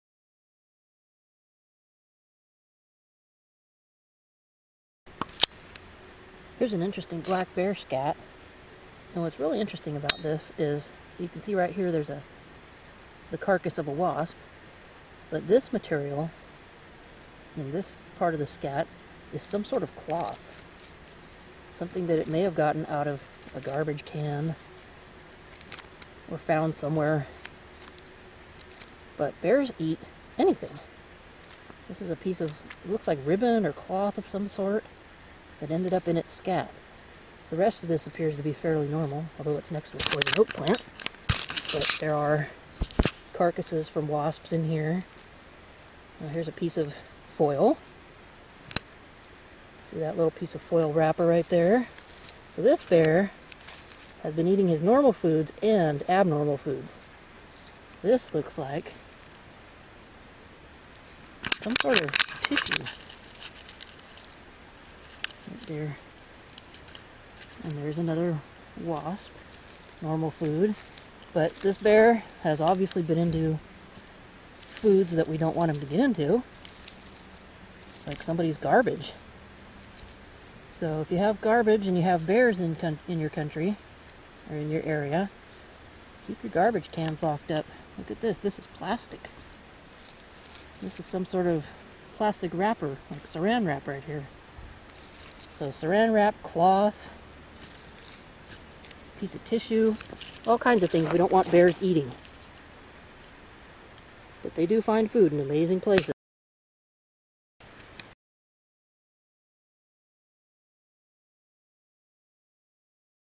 Examining a Black Bear Scat
Take a close look at a black bear scat. What did the bear eat that it shouldn't have?